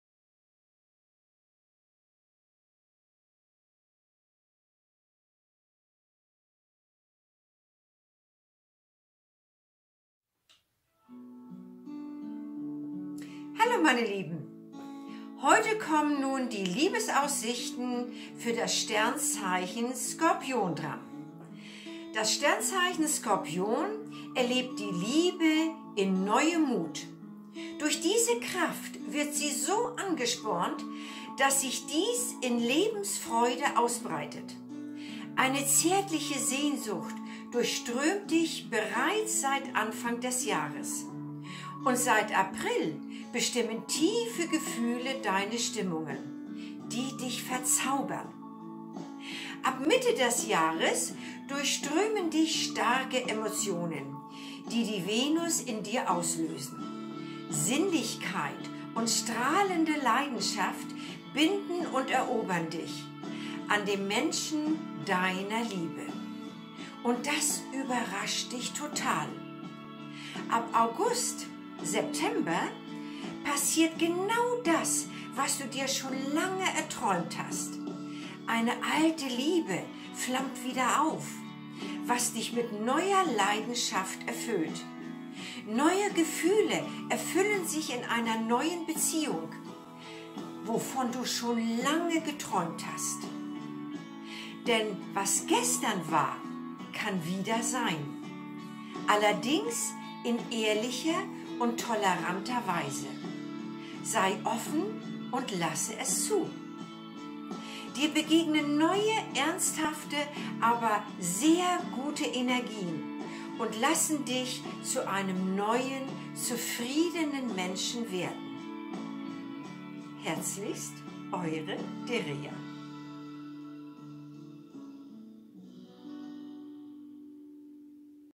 27.10.2017 Das Sternzeichen Skorpion und die Liebe.
Das Sternzeichen Skorpion und die Liebe. Was kommt  in diesem Sonnenjahr 2017 noch auf den Skorpion zu ?

Ich biete an:
*** Hellsichtiges Kartenlegen
*** Astrologie
*** Channeling
*** Coaching
*** Seminare

Seminare:
 *** Engelseminare

Herzlichst
  Deria